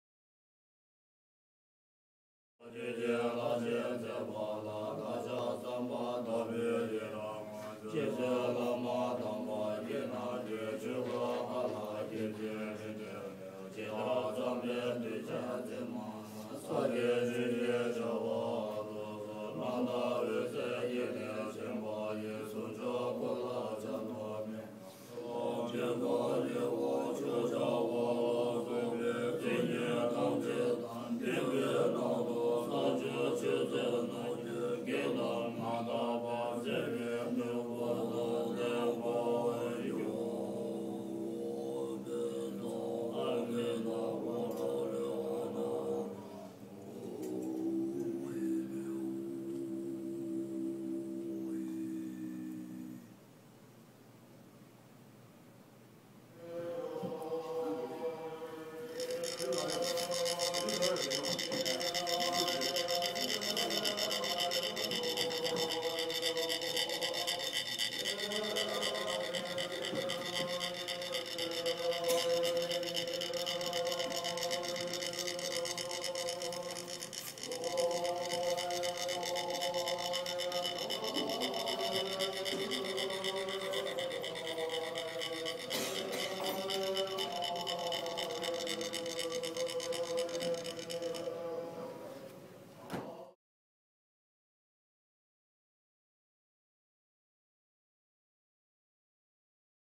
Tibetan Monks Visit Unity of Naples
Purification Ceremony before beginning mandala construction